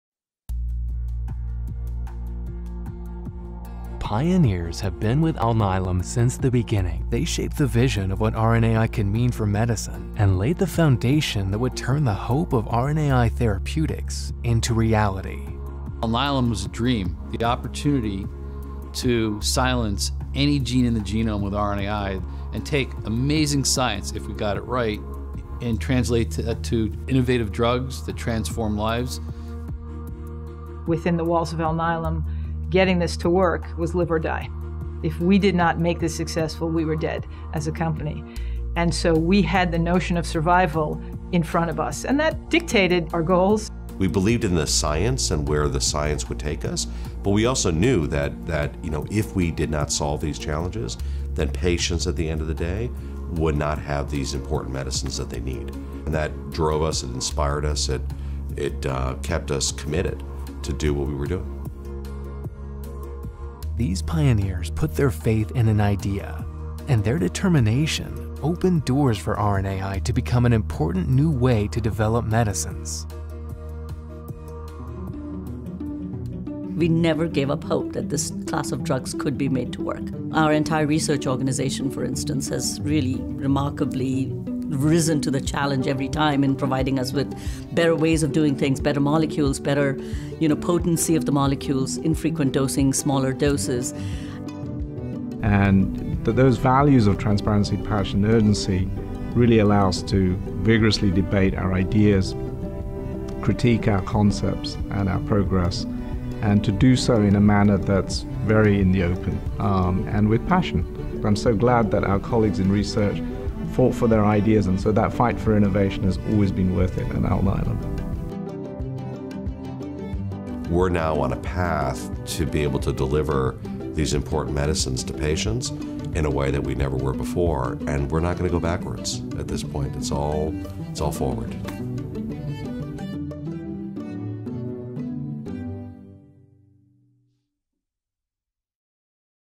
RNA de interferência (RNAi) - Os pioneiros
This video was created for Brazilian audiences.

Os pioneiros estão com a Alnylam desde o começo. Eles moldaram a visão do que o RNAi pode significar para a medicina e lançaram as bases que transformariam a esperança do tratamento com RNAi em realidade.